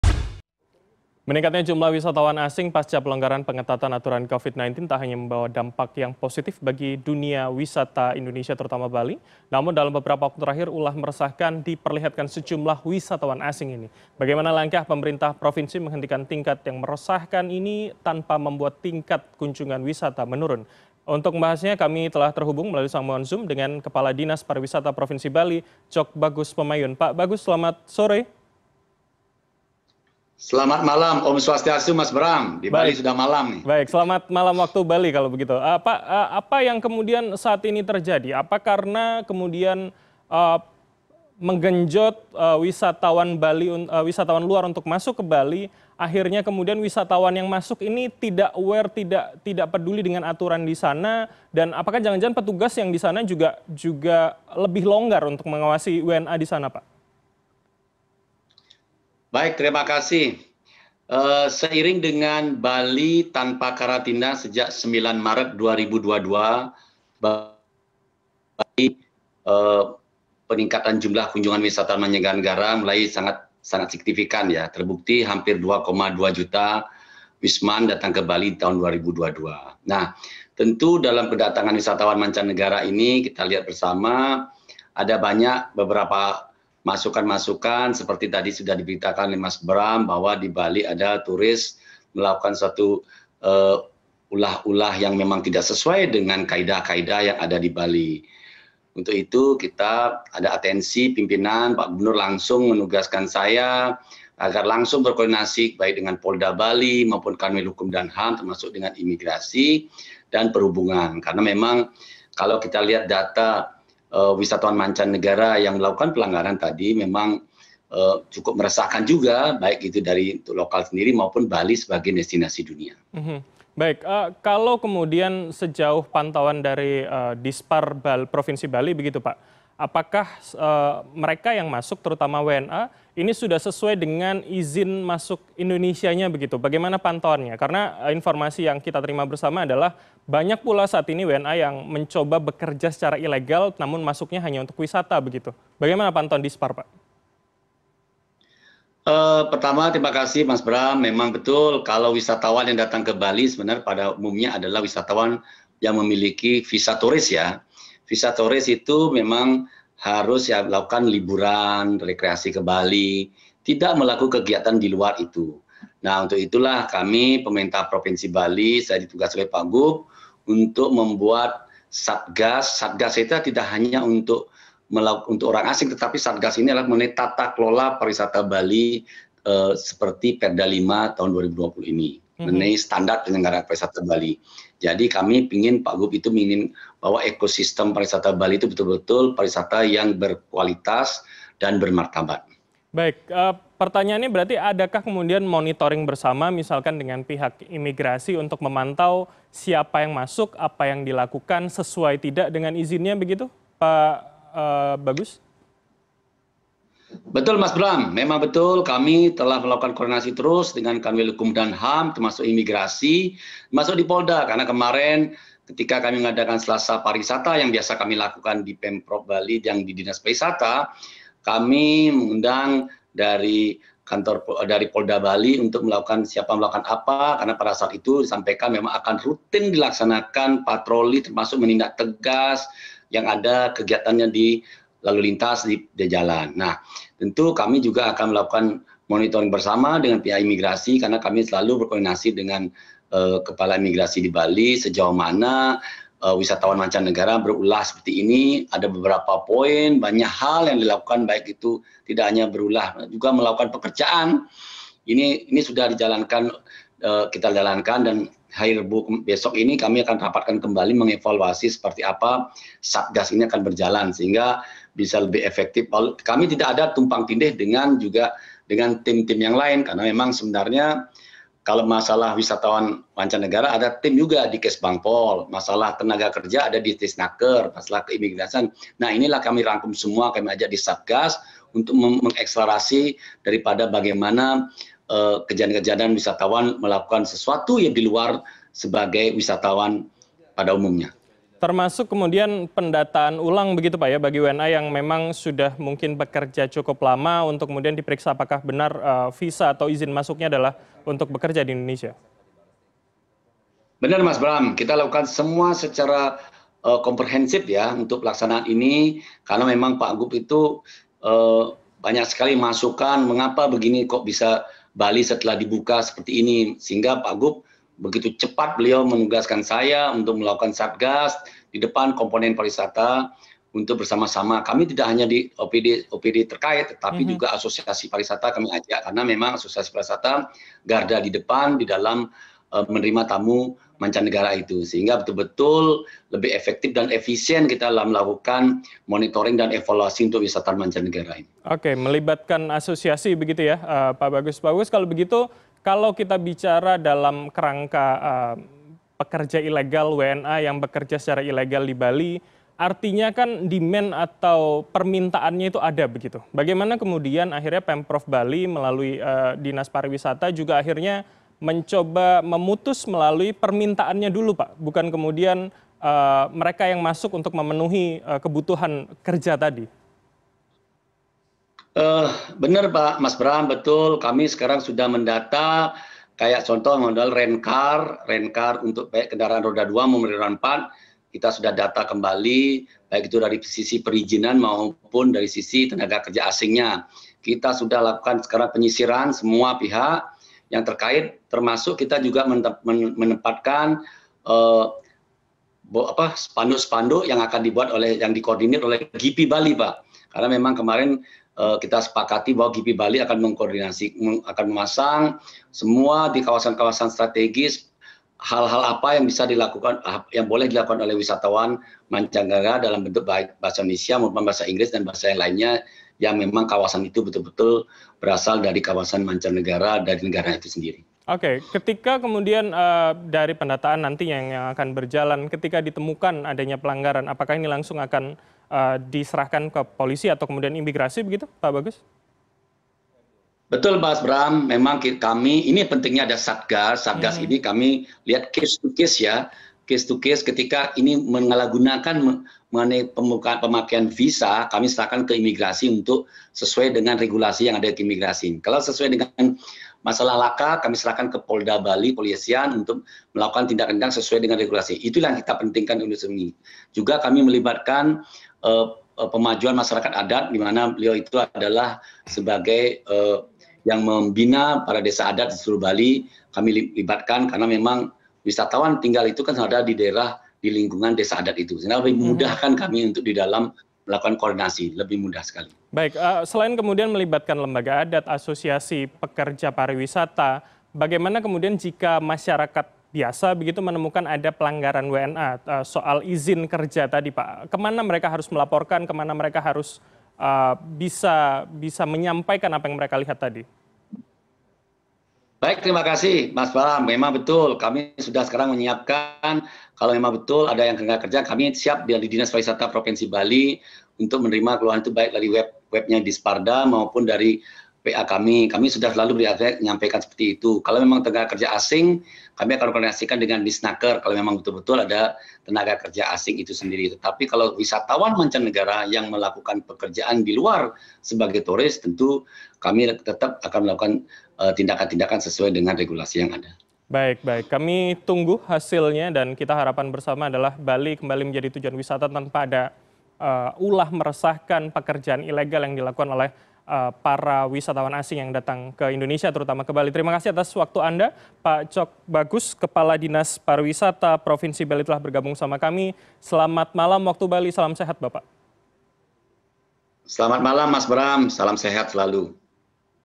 Turis Berulah, Warga Lokal Susah
Meningkatnya jumlah wisatawan asing pasca pelonggaran pengetatan aturan Covid-19 tak hanya membawa dampak postifi bagi dunia wisata Indonesia, namun dalam beberapa waktu terakhir ulah meresahkan diperlihatkan sejumlah wisatawan asing ini. Bagaimana langkah pemerintah provinsi menghentikan tingkah meresahkan ini tanpa membuat tingkat kunjungan wisata menurun? Untuk membahasnya lebih lanjut Bram Herlambang melalui sambungan zoom berbincang dengan Kadis Pariwisata Prov. Bali, Tjok Bagus Pemayun